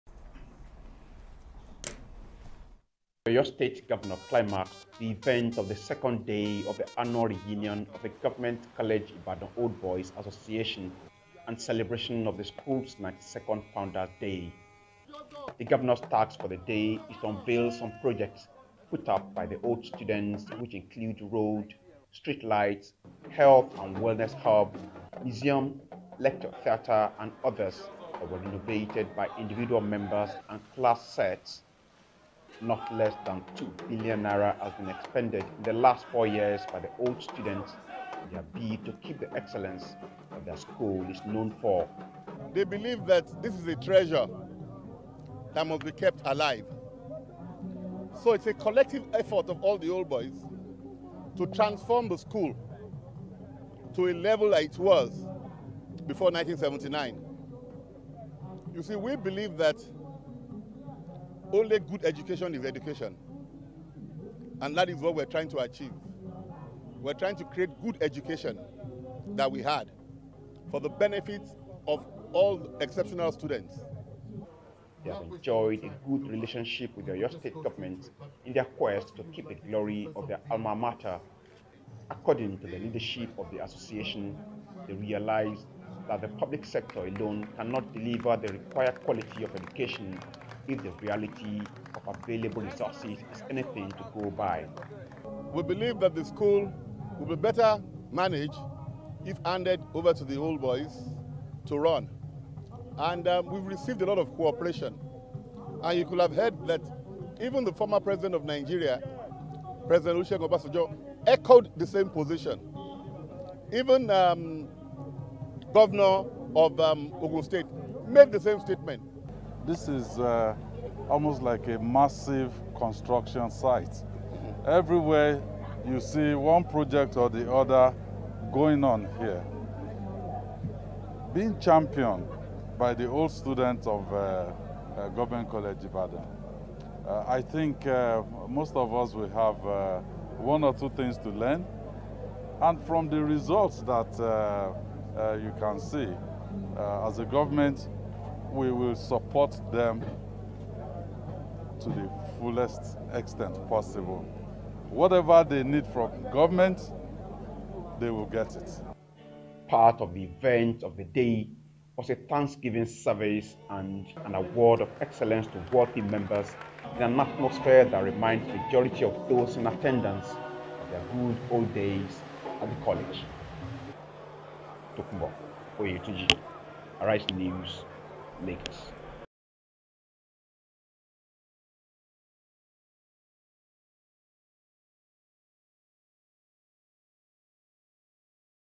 GCI PROJECTS UNVEILING - ARISE NEWS REPORT
Six projects donated by the Government College Ibadan Old Boys Association were unveiled at the premises of the school on Saturday by the Governor of Oyo State, Seyi Makinde. The projects including road, street light, museum, and lecture theatre are part of efforts by the old boys to keep the legacy of excellence that the GCI is known for.
Tokunbo Oyetunji has the details.